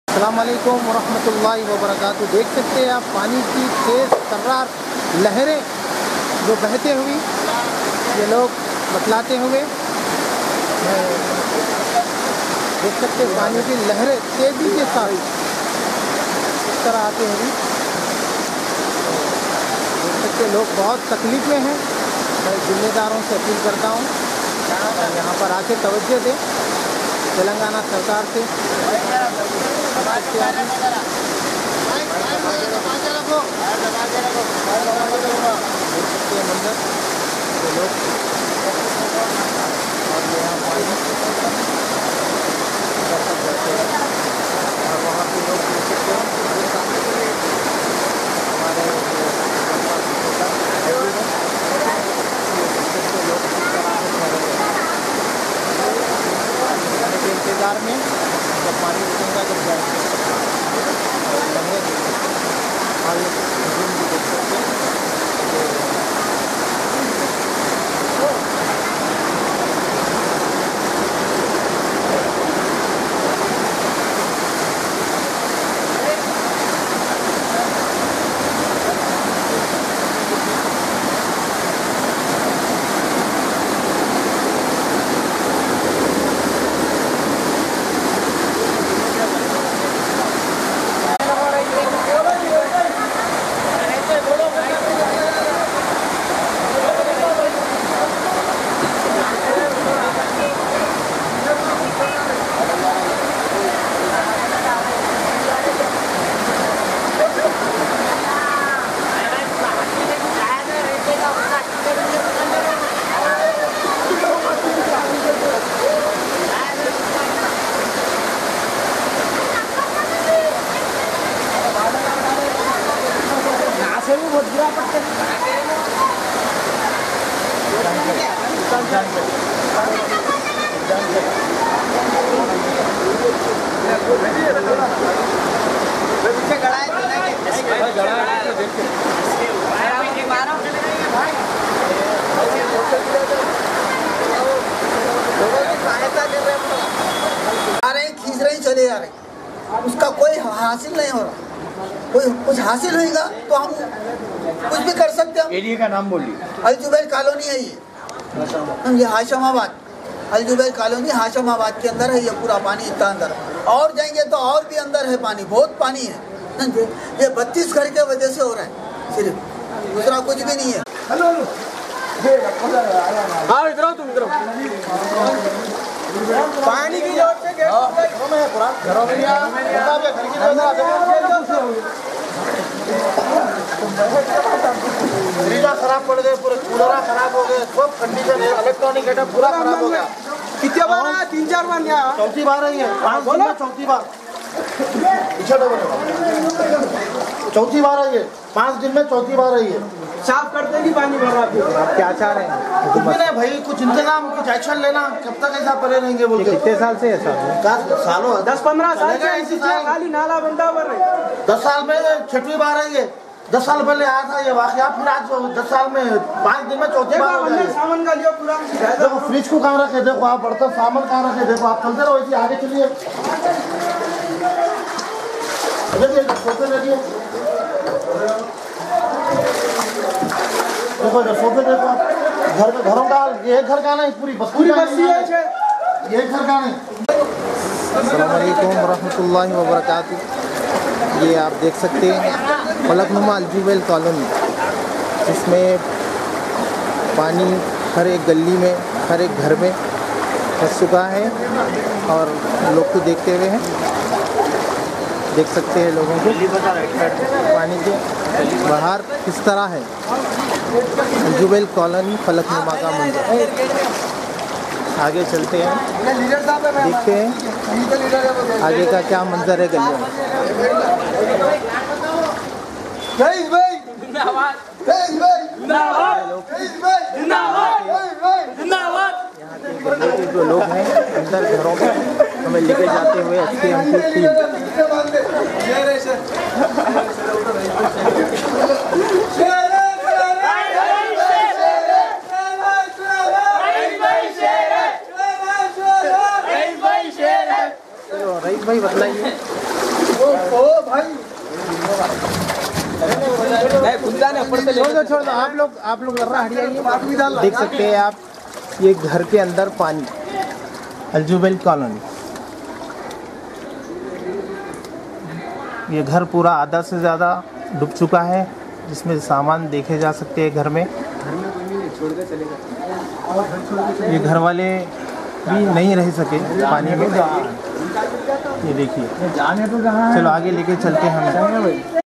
Falaknuma Al jubail cly me barish ke pani ne tabahi machadi. Koyi madad leader aur na officail ayaa
TV21NEWS CHANNEL HYDERABAD